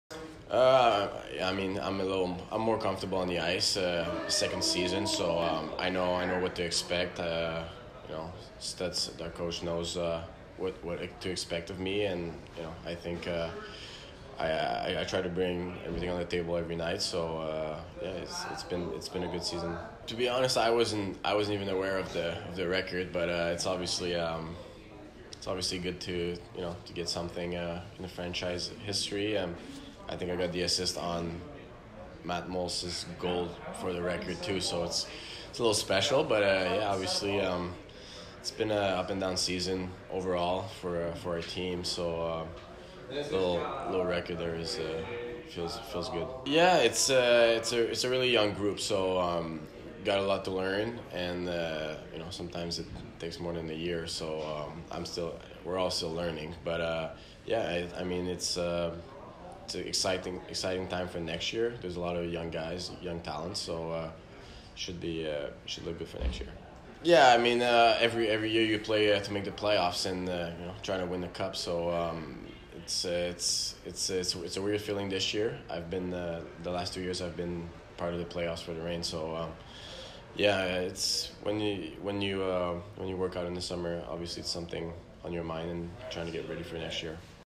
Philippe Maillet Post Season Interview
Forward Philippe Maillet talks about setting the Reign's single-season assist mark and more in his post-season interview.